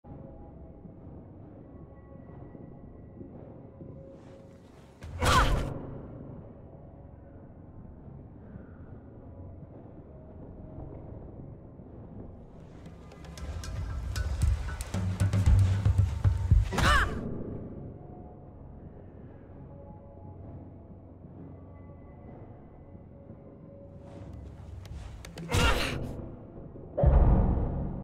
When the ranged DPS has personal space issues
I figured he wanted a hug considering he had his arms out but apparently not.